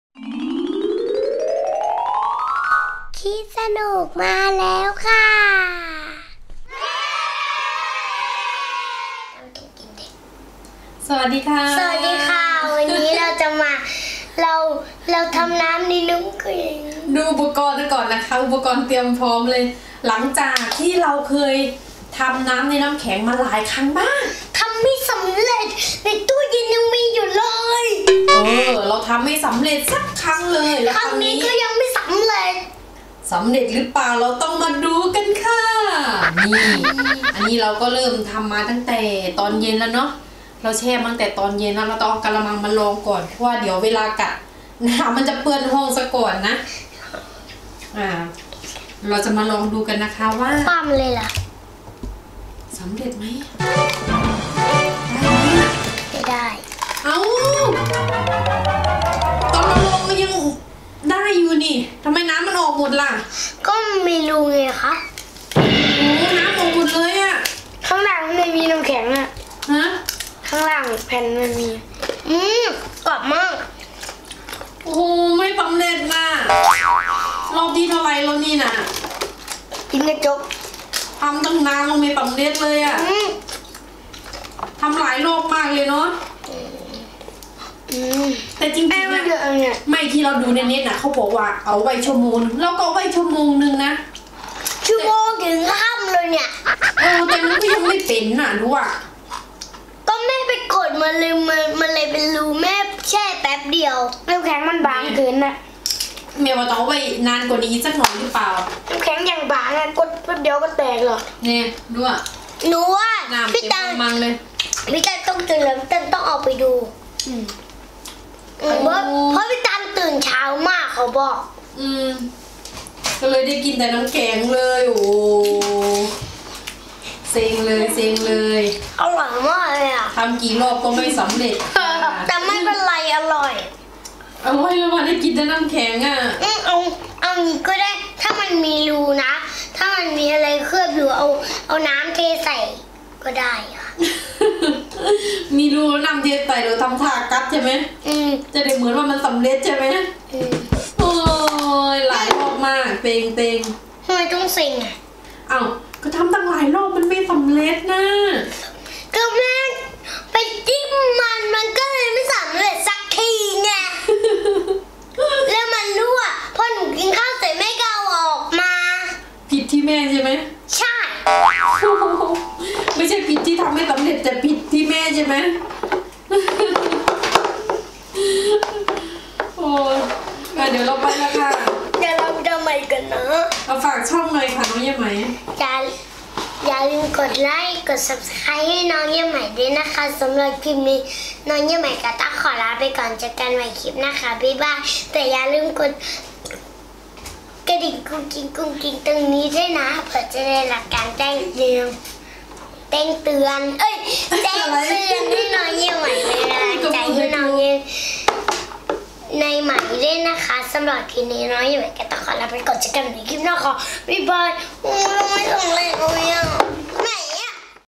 ลองทำน้ำในน้ำแข็ง l น้องใยไหม kids snook
ลองทำหลายรอบมากแต่ไม่สำเร็จสักที
น้ำในน้ำแข็ง